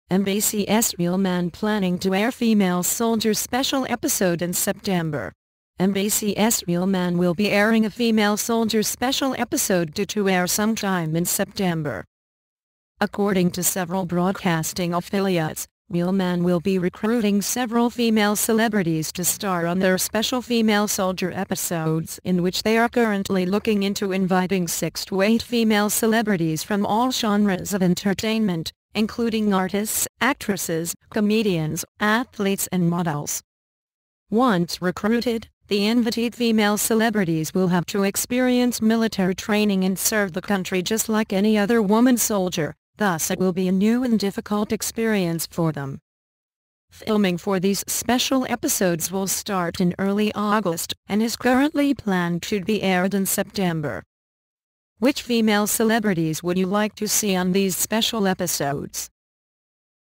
MBC'S "REAL MAN" PLANNING TO AIR "FEMALE SOLDIER" SPECIAL EPISODE IN SEPTEMBER
MBC's "Real Man" will be airing a "female soldier" special episode due to air some time in September.

According to several broadcasting affiliates, "Real Man" will be recruiting several female celebrities to star on their special "female soldier" episodes, in which they are currently looking into inviting 6 to 8 female celebrities from all genres of entertainment, including artists, actresses, comedians, athletes and models.

Once recruited, the invitied female celebrities will have to experience military training and serve the country just like any other woman soldier, thus it will be a new and difficult experience for them.

Filming for these special episodes will start in early August, and is currently planned to be aired in September.

Which female celebrities would you like to see on these special episodes?